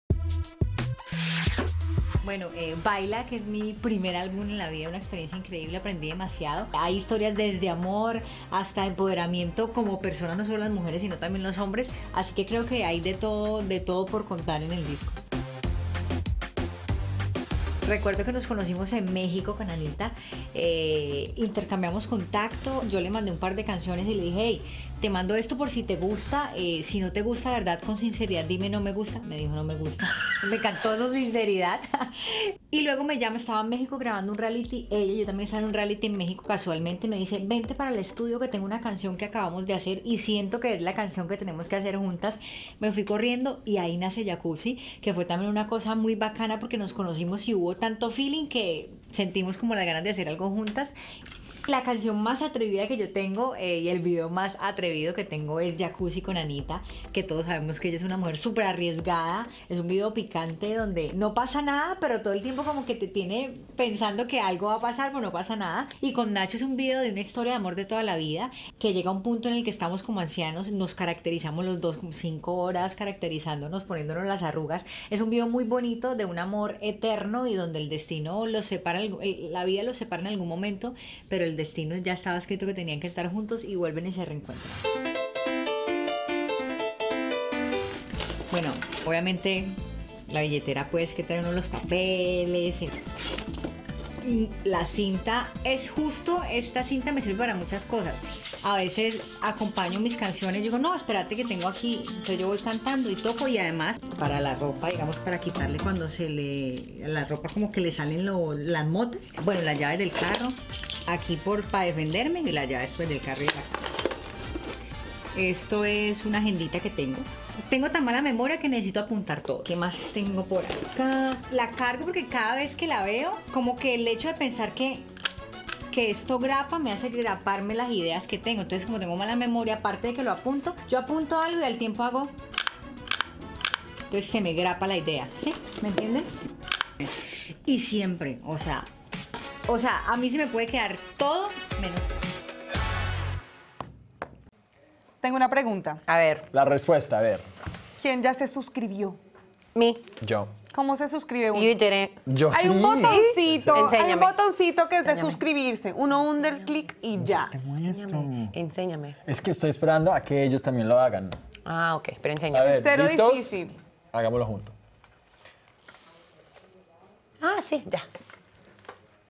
Greeicy comparte detalles de su sensual video con Anitta | Latinx Now! | Entretenimiento
Latinx Now! Entrevistamos a la cantante colombiana a raíz del lanzamiento de su primer álbum y nos contó detalles de su sensual video musical con Anitta. Conoce qué lleva Greeicy en su bolso de mano, no te imaginas lo que carga con ella siempre.